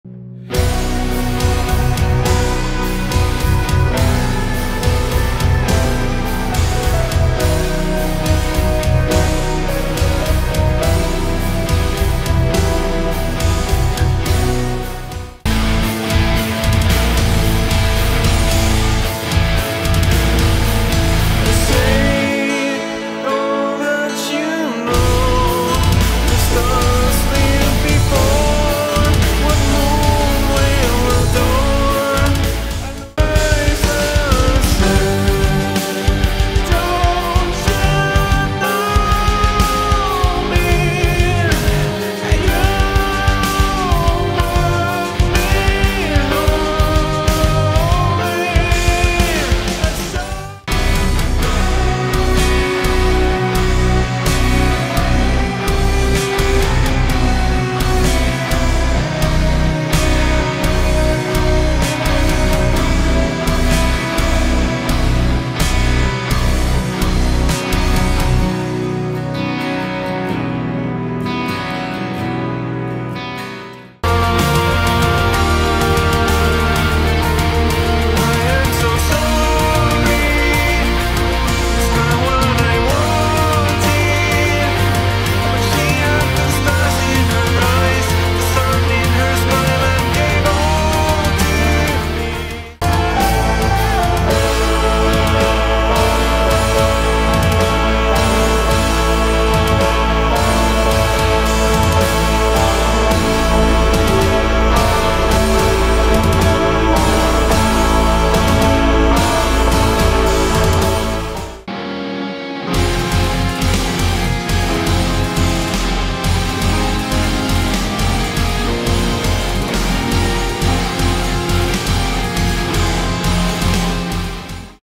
Ilkka Malassu 2022 - 2023 Progressive Metal TLDL
Highlights of the past year in Ilkka Malassu music.